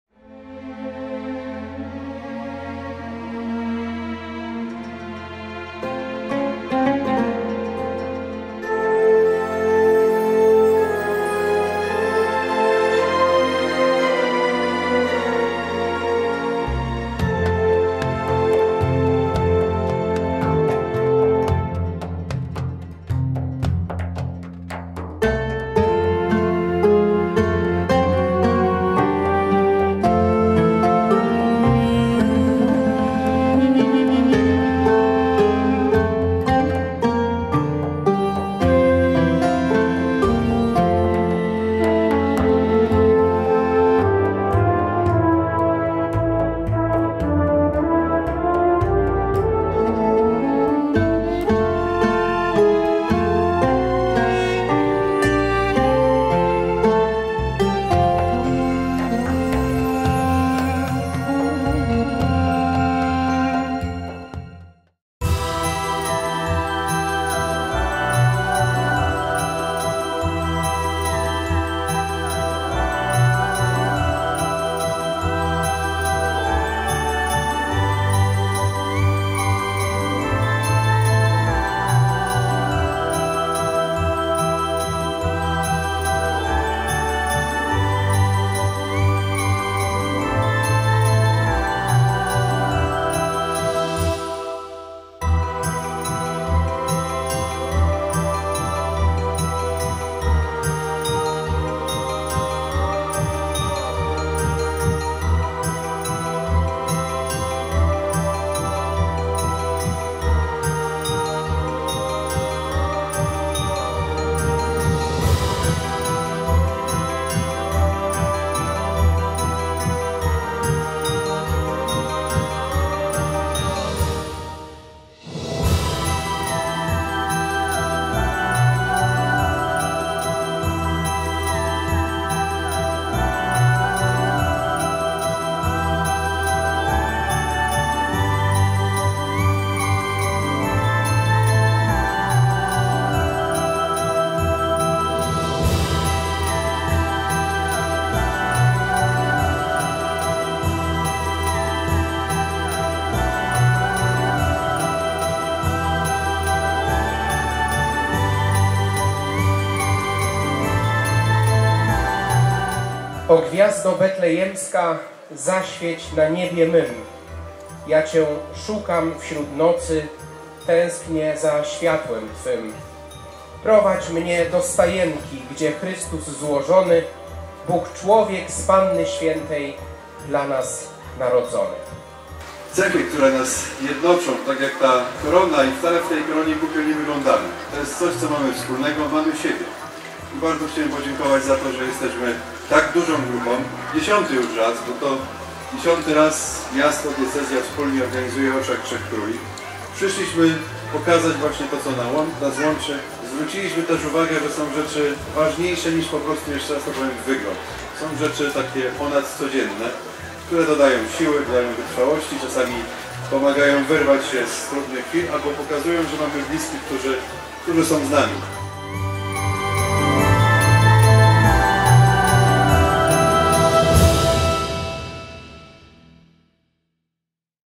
Orszak Trzech Króli 2023
Ulicami Opola przeszedł dziś Orszak Trzech Króli. Mimo niepogody mieszkańcy dopisali i tłumie uczestniczyli w barwnym korowodzie. Królom towarzyszyli Józef z Maryją, Gwiazda Betlejemska, aniołowie, harcerze, strażacy i orkiestra dęta Zespołu Szkół Żeglugi Śródlądowej z Kędzierzyna-Koźla.